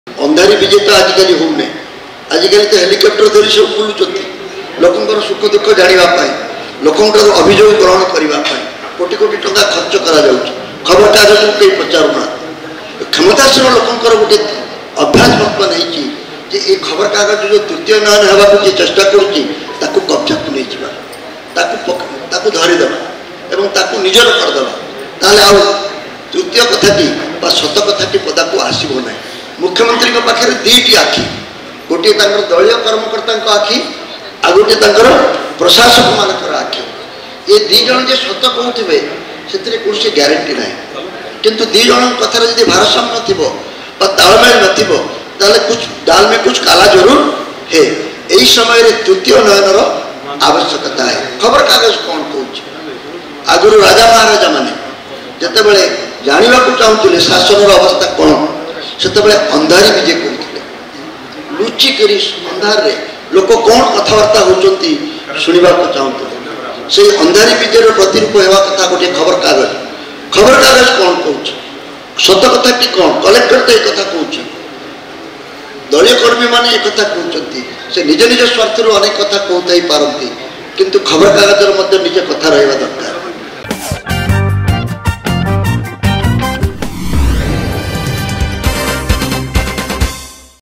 ‘ନେତା ହେଲିକପ୍ଟରରେ ବୁଲି ଅନ୍ଧାରୀ ବିଜେ କରୁଛନ୍ତି’ || Soumya Ranjan Patnaik || Odisha Reporter
କ୍ଷମତାଶୀଳ ଲୋକଙ୍କ ଏକ ଅଭ୍ୟାସ ହୋଇଯାଇଛି ତୃତୀୟ ନୟନକୁ କବଜା କରି ରଖିବା। ଯାହା ଫଳରେ ସତକଥାଟି ପଦାକୁ ଆସିବ ନାହିଁ। ଶୁଣନ୍ତୁ ଆଉ କ'ଣ କହୁଛନ୍ତି ସୌମ୍ୟରଞ୍ଜନ ପଟ୍ଟନାୟକ...